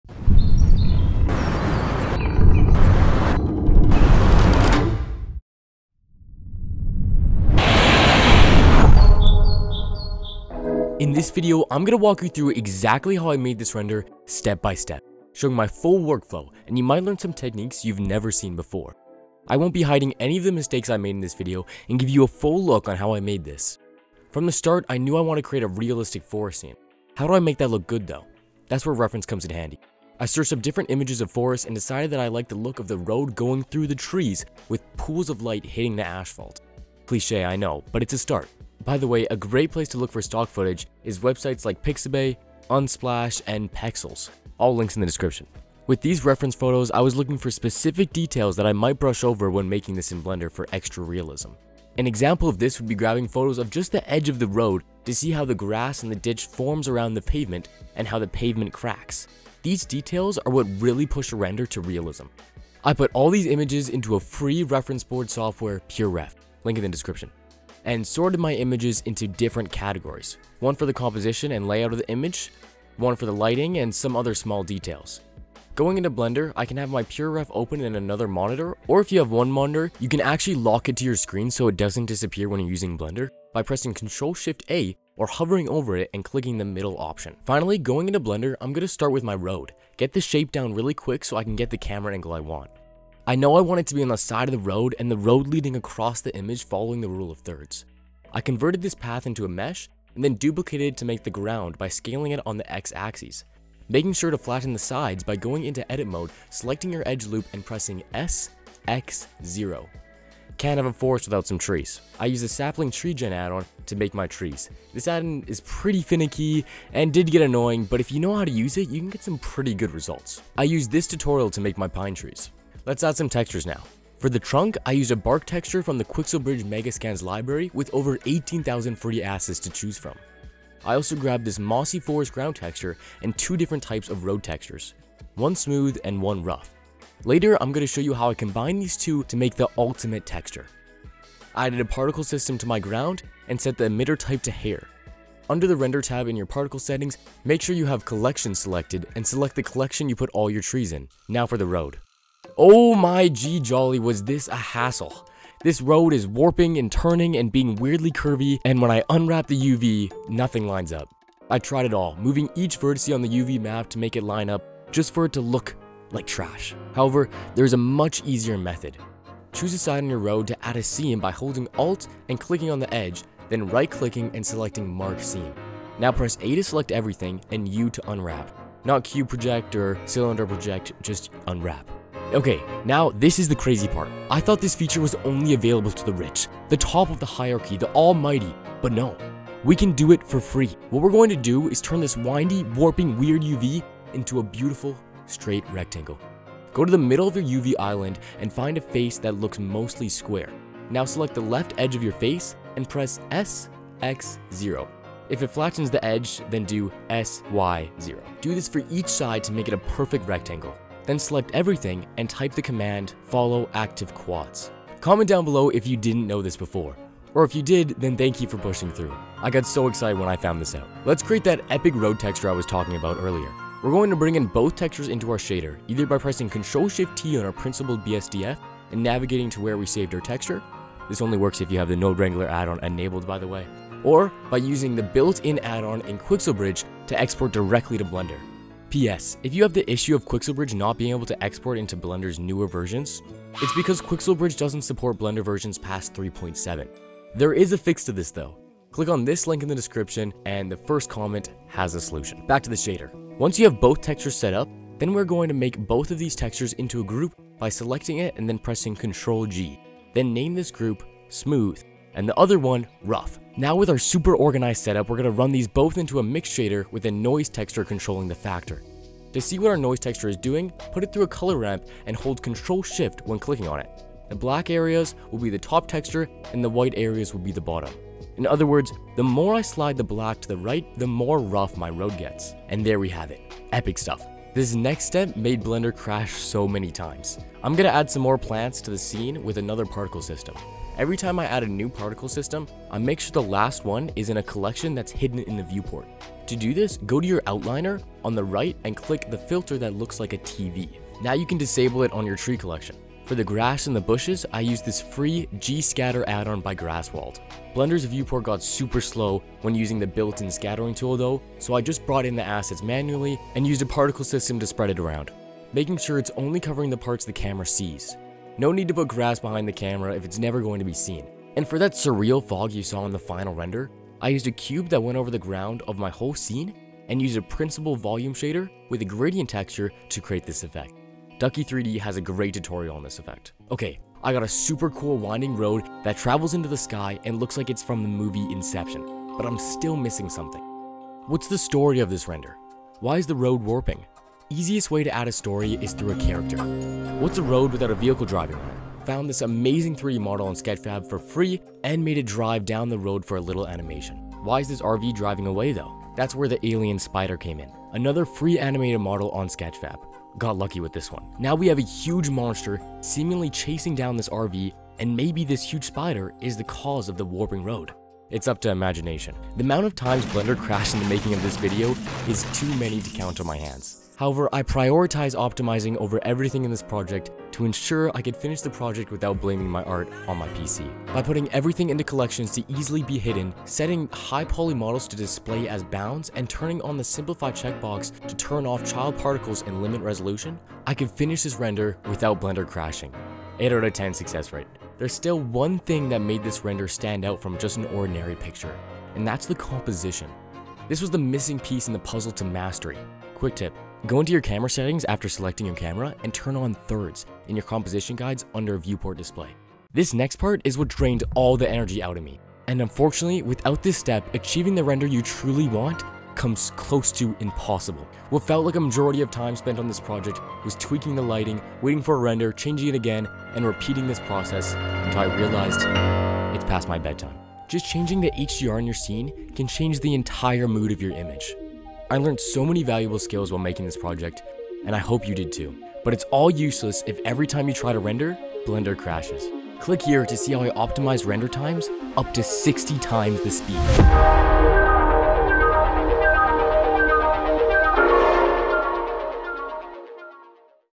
Full Workflow: Epic Villain Chase in Blender | 2024 Breakdown
🎬 *How I Created a Realistic Forest Render in Blender* 🌲

In this video, I walk you through my entire process of creating a realistic forest scene in Blender, sharing every step and mistake along the way. You'll see my full workflow, from gathering references to optimizing render times.

0:00 - Introduction
0:24 - Gathering References
1:02 - Organizing with PureRef
1:27 - Creating the Road
1:52 - Generating Trees with Sapling Tree Gen Addon
2:31 - Advanced UV Mapping Techniques
4:12 - Road Texture
4:44 - Extra Foliage
5:21 - Fog
5:33 - Adding Story Elements with Free 3D Models
6:09 - Optimizing Scene Performance
6:43 - Composition and Lighting Tips
7:08 - Final Thoughts

🔗 *Resources & Links:*

Stock footage

Your feedback helps me create content you want to see. Drop a comment with your questions or video requests.